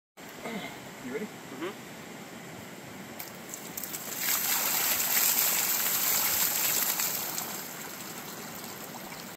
Dowsing at Survival Camp for Teenagers 2024
Dowsing is an ancient technique to learn to relax and adapt the body to stresses not normally experienced in day-to-day living. The teens are coached using breath and muscle relaxation to prepare for the dowsing of cold water.

As the cold water comes in contact with the body the blood retreats to the body's core, its way of protecting the extremities. As the blood returns a relaxing and near euphoric experience may occur.

There are some schools of thought that dowsing may boost the immune system. Regardless, our teens toughen up and get grit with this early morning exercise. Good job!